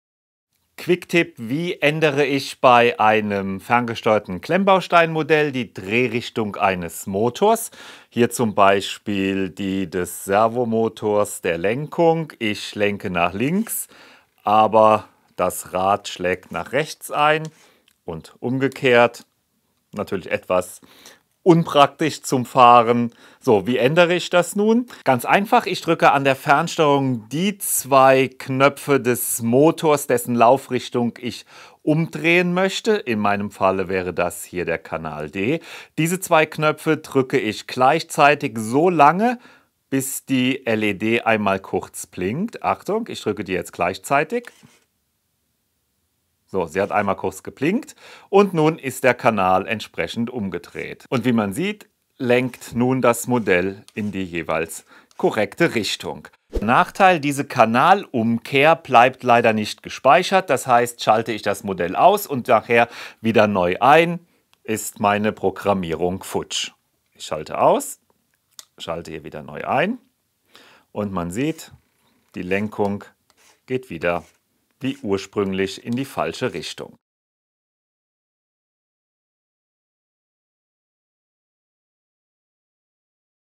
Motorlaufrichtung via Fernsteuerung ändern: CADA, MOULD KING, REOBRIX, LEGO® etc...
Die Drehrichtung eines Motor bei ferngesteuerten Klemmbausteinsets lässt sich ganz einfach mit der Fernsteuerung ändern. In diesem Video zeige ich wie's geht...